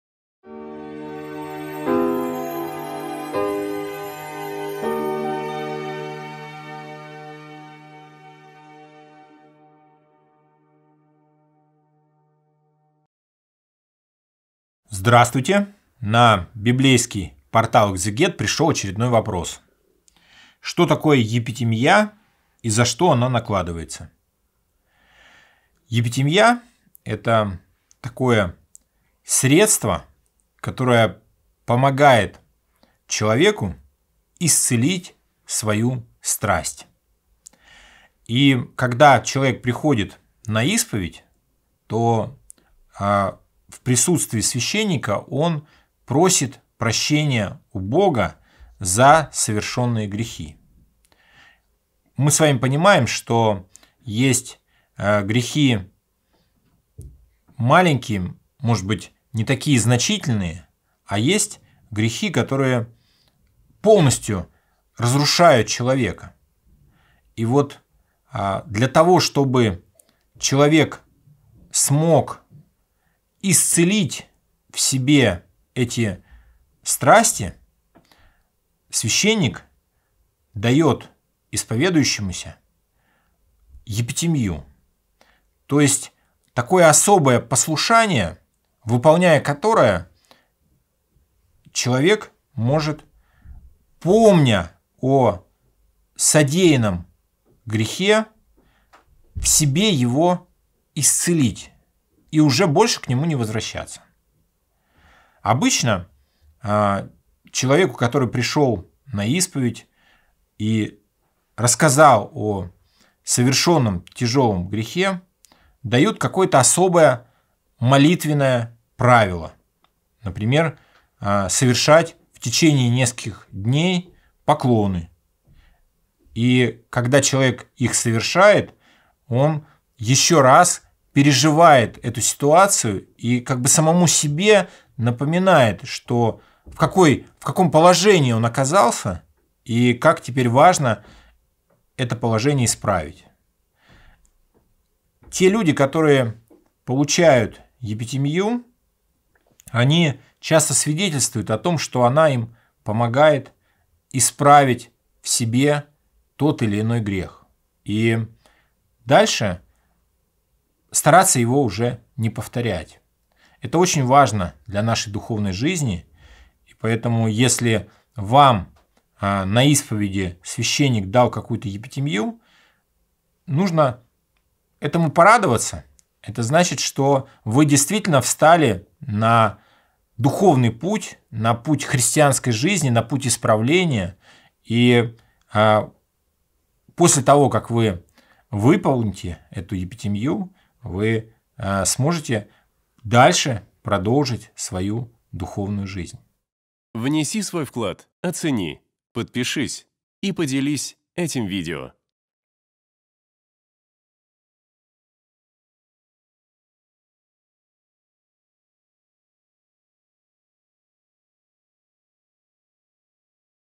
ЧТО ТАКОЕ ЕПИТИМЬЯ ? Протоиерей Виктор Горбач
Команда Экзегет.

В сегодняшнем видео на библейском портале Экзегет, протоиерей Виктор Горбач отвечает на вопрос: "Что такое епитимья?"

🔸  Разрушающий грех
🔸  Покаяние
🔸  Исцеление страсти
🔸  Изменение жизни